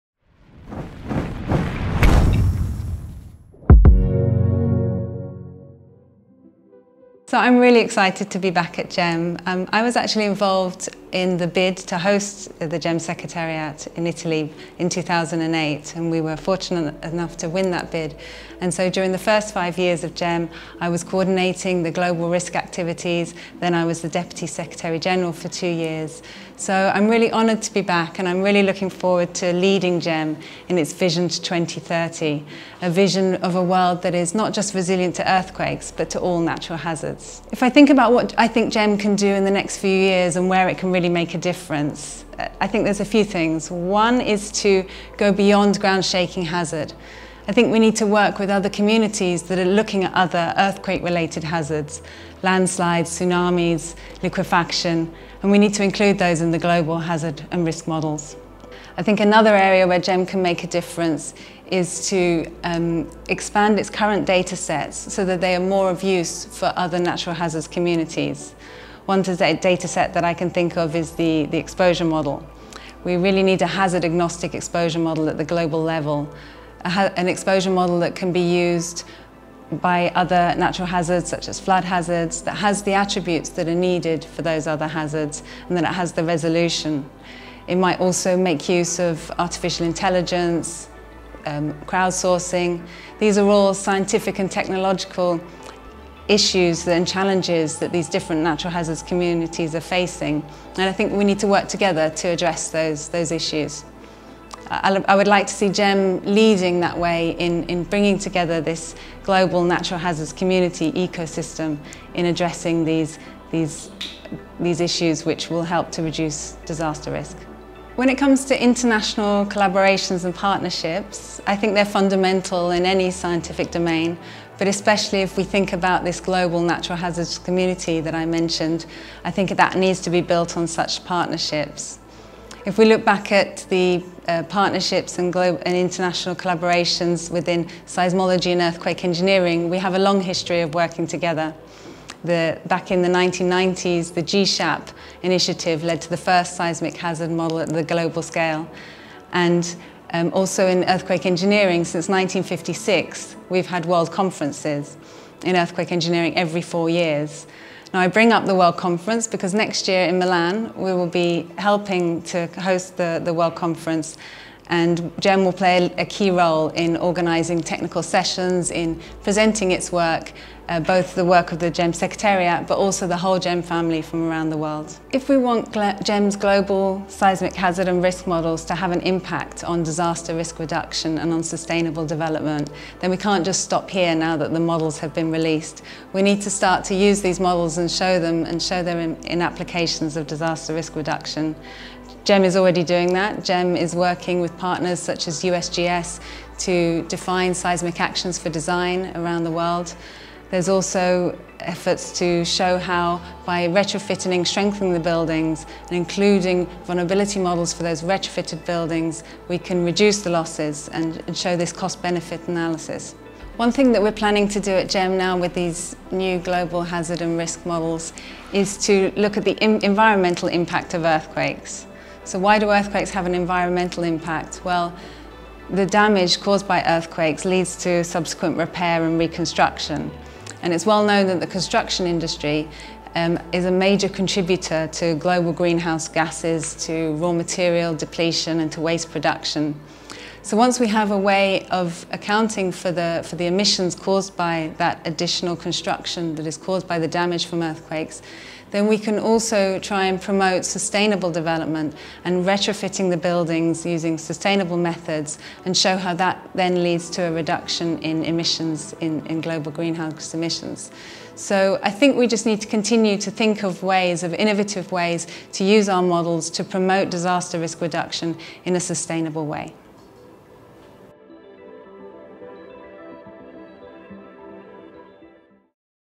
Helen Crowley: New GEM Secretary General
In an interview with Helen Crowley, the new Secretary General of the Global Earthquake Model (GEM) Foundation, we delve into the organization's ambitious vision for a world resilient to natural hazards, not just earthquakes. The new leader shares insightful perspectives on GEM's potential to make a difference, expand its impact, foster partnerships, and promote sustainable disaster risk reduction.

The new GEM Secretary General's vision reveals a multifaceted strategy, focused on embracing a multi-hazard approach, strengthening partnerships, and ensuring the practical application of GEM's models for sustainable disaster risk reduction. By venturing beyond conventional boundaries, GEM endeavors to create a resilient world for all natural hazards.